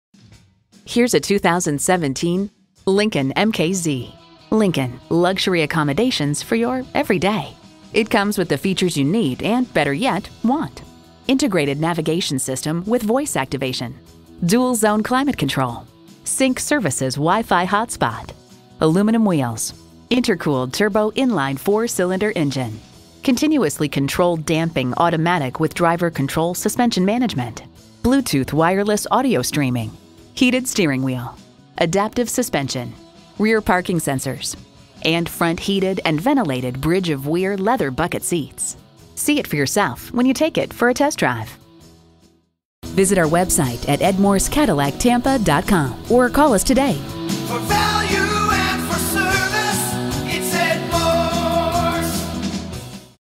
Used 2017 Lincoln MKZ Tampa FL St. Petersburg, FL R0102408A - SOLD
Year: 2017
Make: Lincoln
Model: MKZ
Trim: Black Label
Engine: 2.0Ll 4 Cylinder Turbocharged
Transmission: FWD 6-Speed Shiftable Automatic
Interior: Thoroughbred
Mileage: 62419
Stock #: R0102408A
VIN: 3LN6L5G97HR633833

Address:
101 East Fletcher Avenue